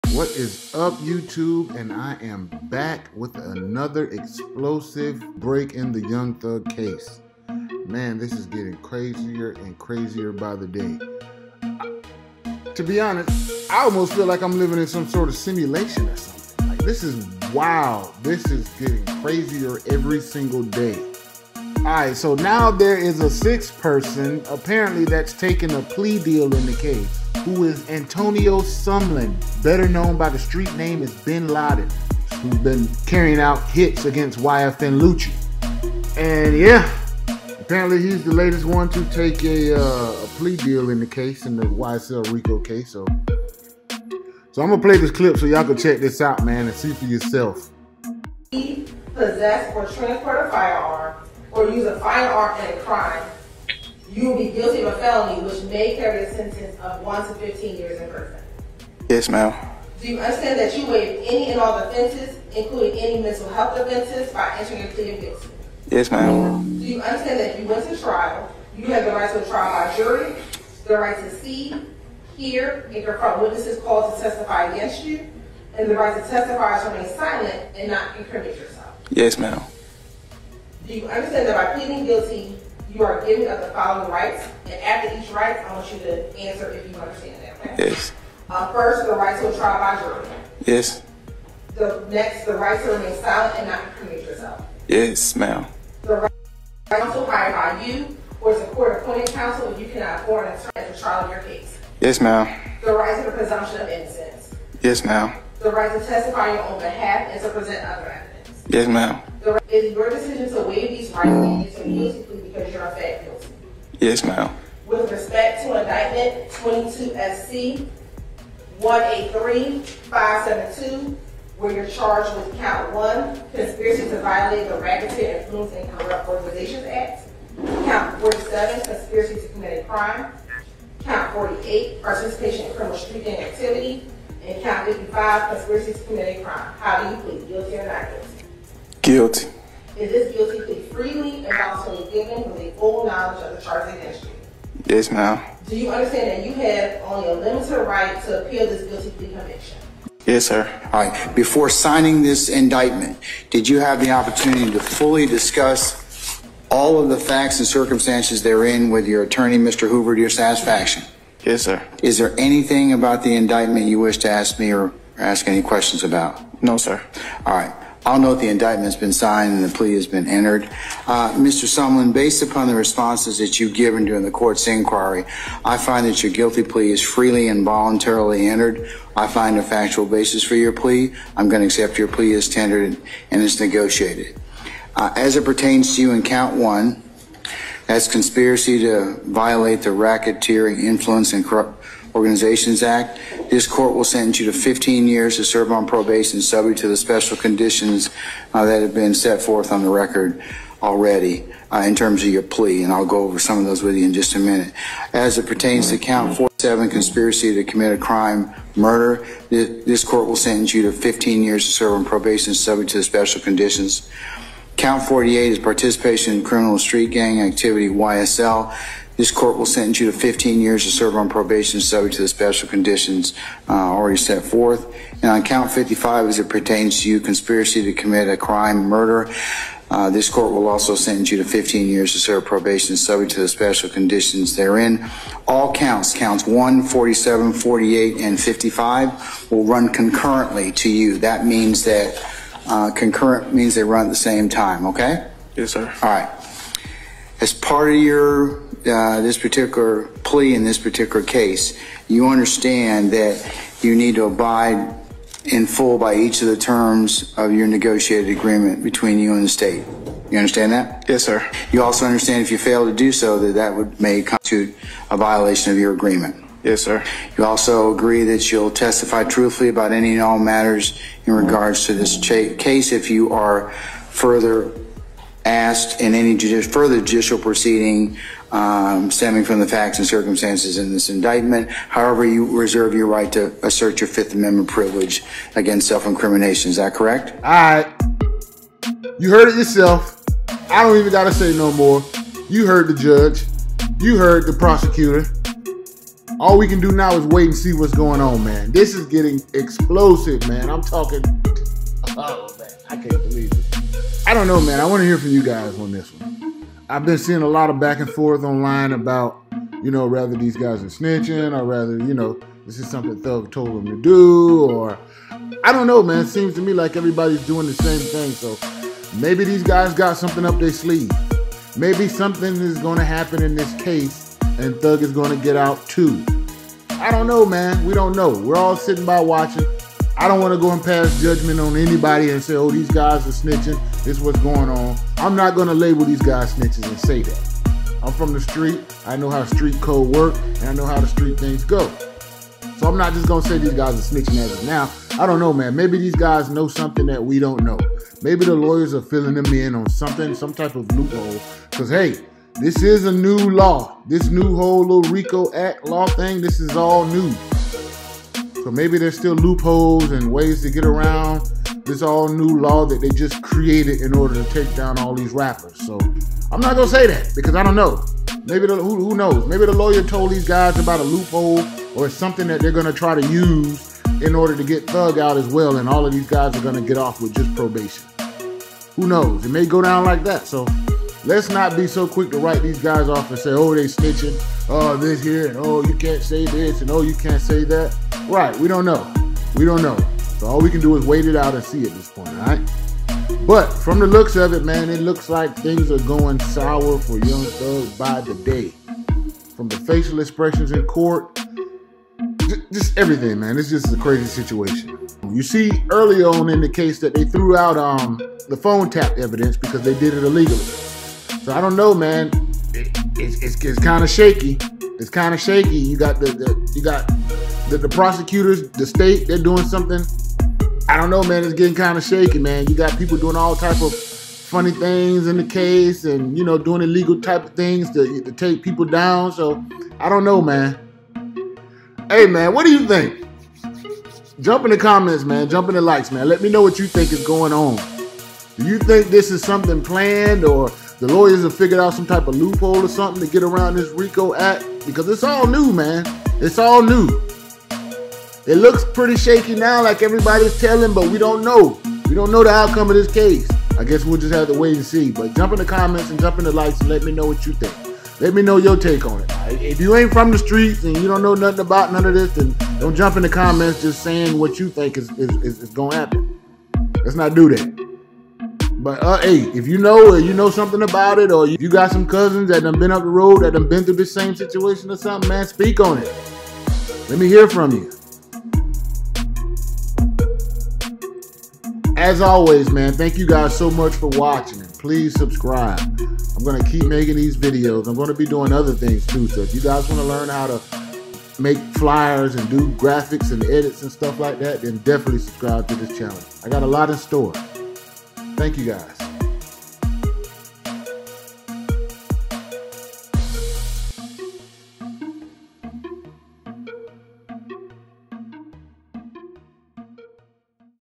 YSL "THE LABEL"? or "THE GANG"?
The Case against YSL Records is Unfolding by the day..today marks another member, who is now the 6th person to enter a plea deal in the case. this time the guy is young thugs alleged hitman Obama who enters the plea deal!